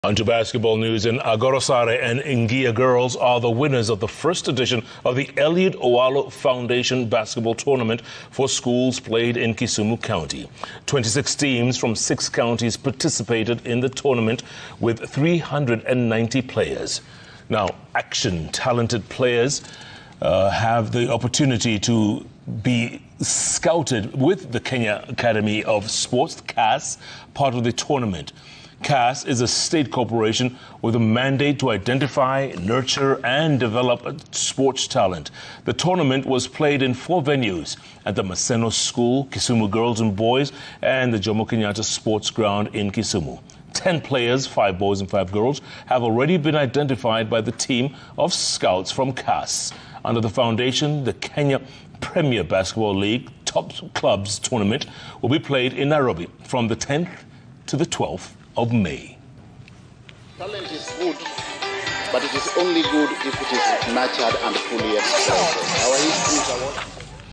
Agoro Sare, Ngiya Girls win Eliud Owalo's inaugural basketball tournament
Agoro Sare And Ngiya Girls Win Inaugural Tournament
Ten Players Identified By Cas At The Weekend Event